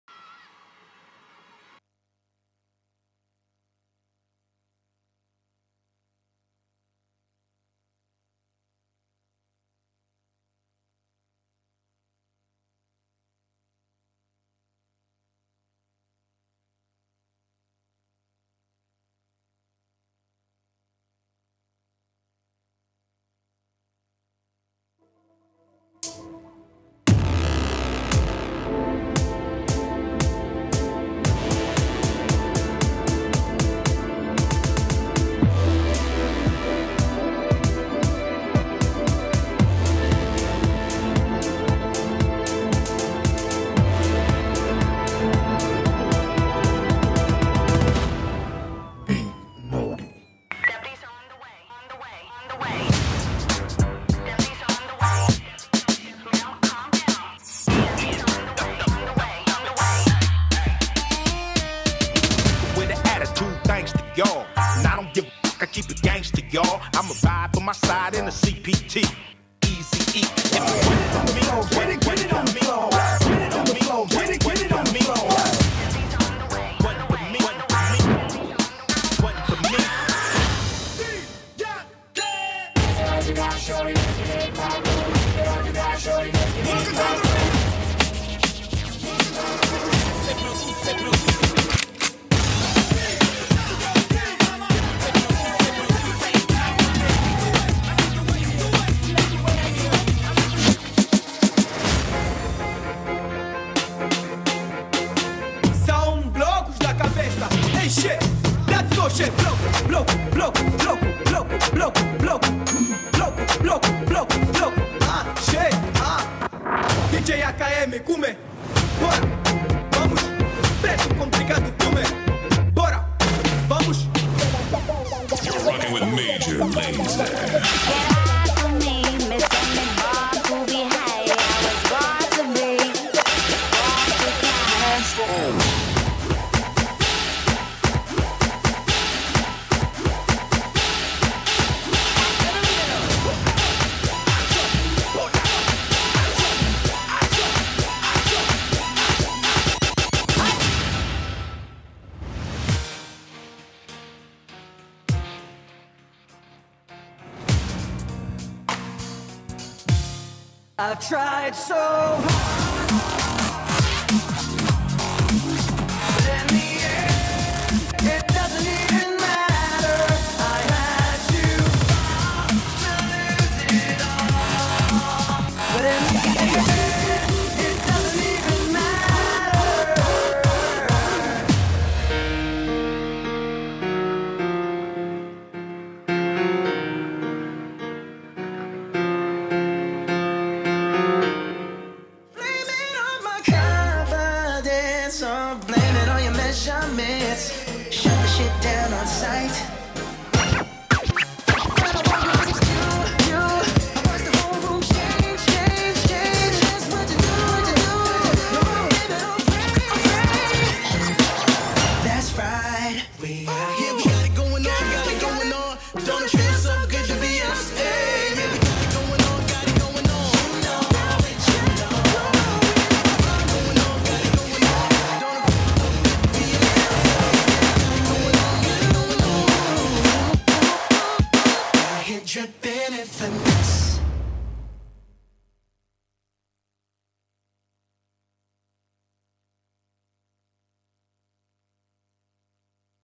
BIG NAUGHTY | Semifinales JV MEGACREW Clasificatoria | HHI Spain 2024
BIG NAUGHTY - SEMIFINALES Categoria JV MEGACREW Clasificatoria en HHI SPAIN 2024

Hip Hop International Spain 2024, Olimpic Arena de Badalona, Barcelona, 27 y 28 de Abril de 2024

Más de 250 grupos de toda España se reúnen en Barcelona para competir por un puesto en el mundial de Hip Hop International en Phoenix Arizona